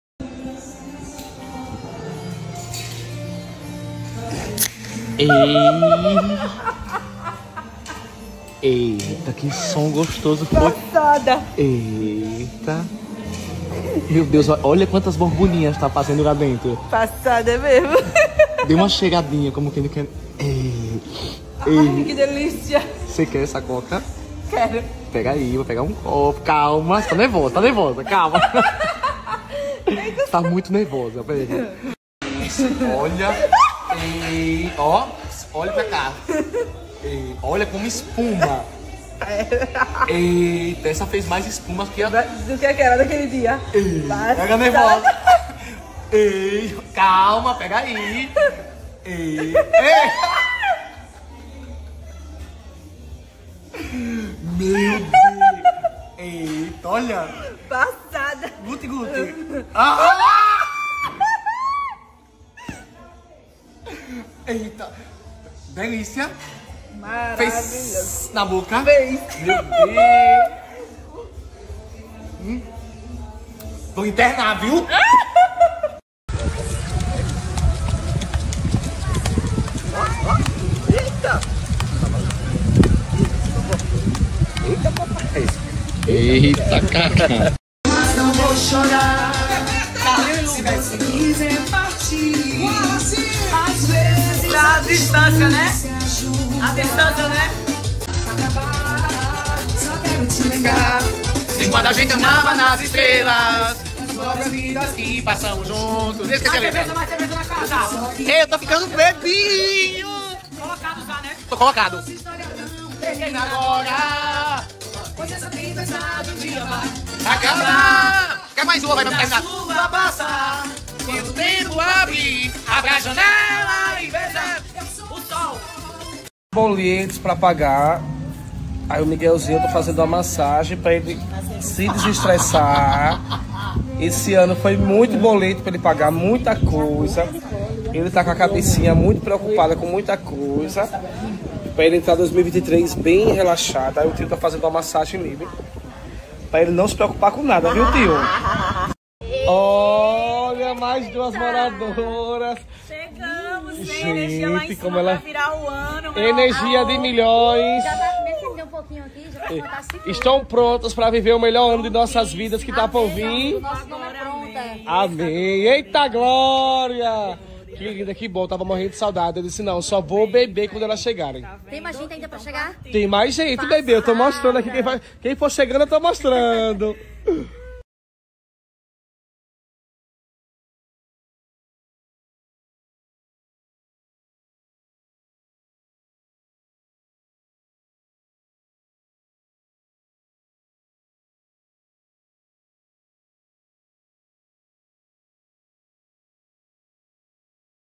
🌻 CARLINHOS MAIA FEZ UMA SURPRESA PRA BABÁ DE EMILY GARCIA 😱😍😱
olha só o que o Carlinhos Maia resolveu fazer com a babá de Emily Garcia e Babau Guimarães 🤭🤣🤭🤣🤭🤣🤣🌻🌻😍🌻🌻